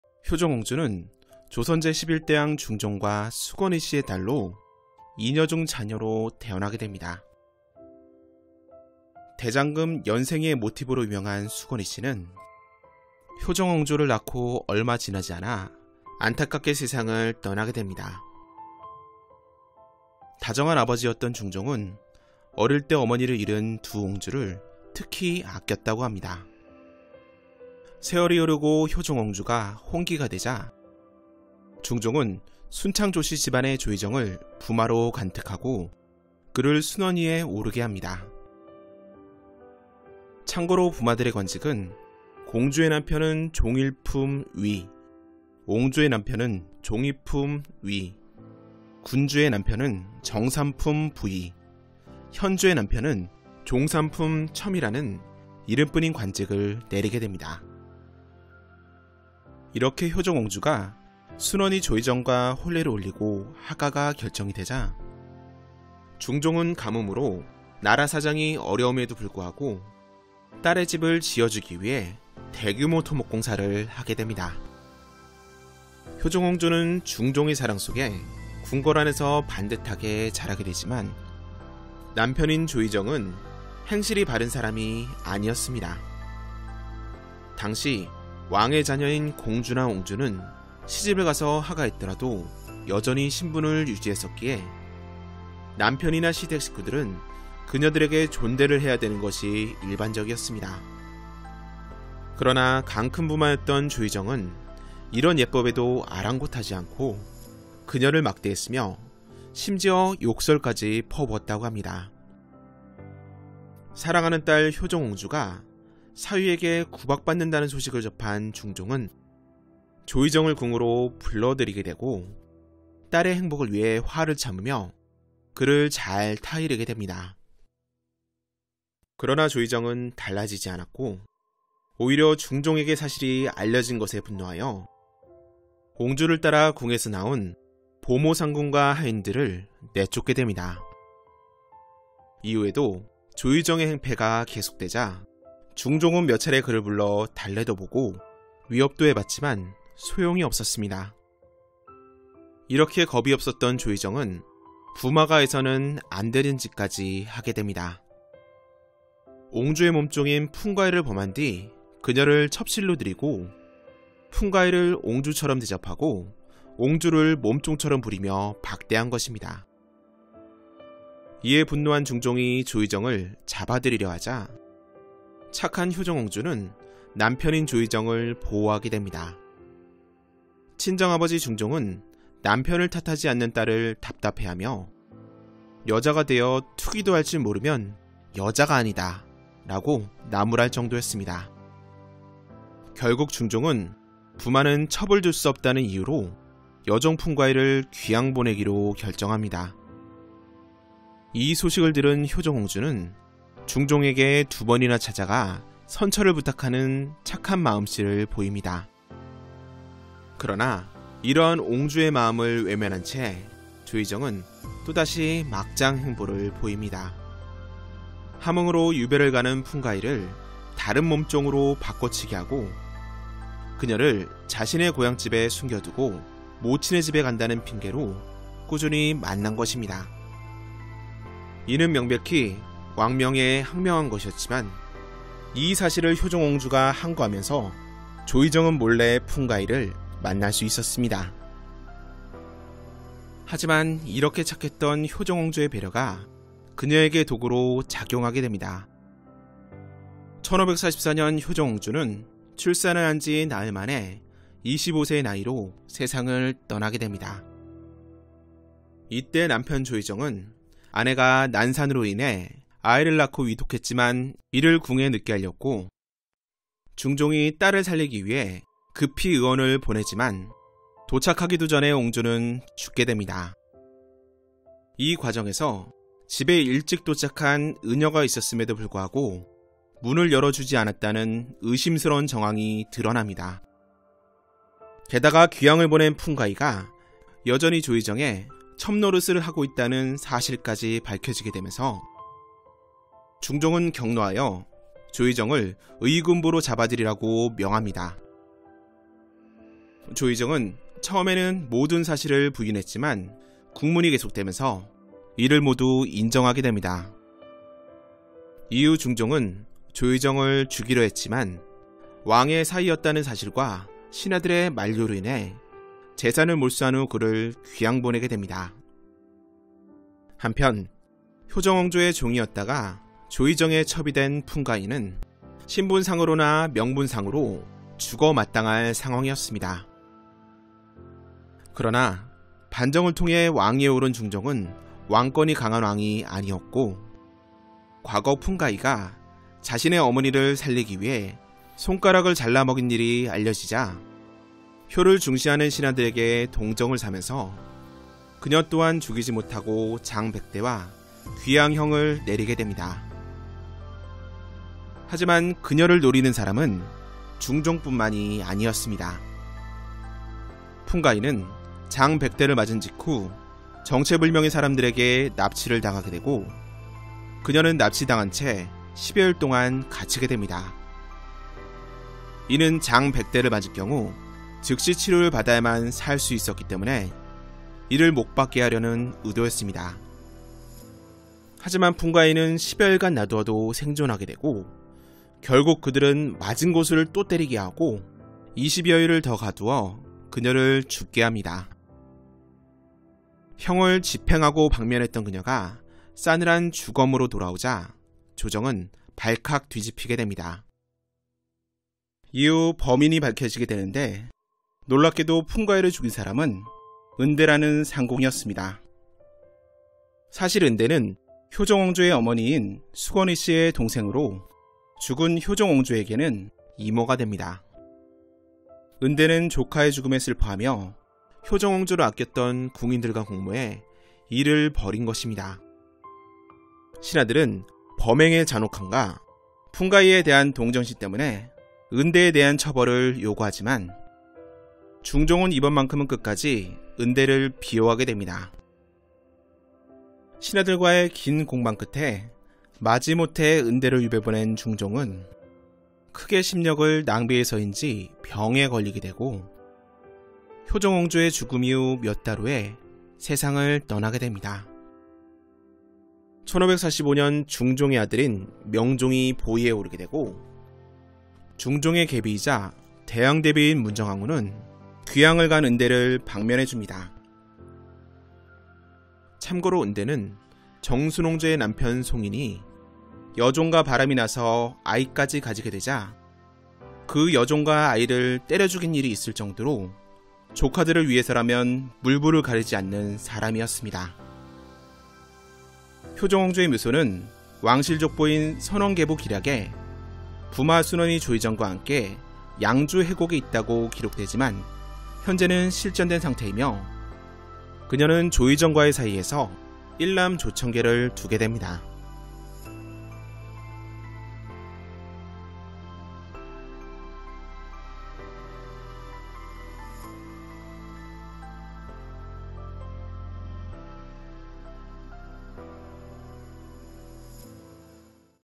조선시대 비극적인 운명의 왕녀 효정옹주
효정옹주(1520년~1544년)는 조선 제11대 왕 중종과 숙원 이씨의 딸로 2녀 중 차녀로 태어나게 됩니다.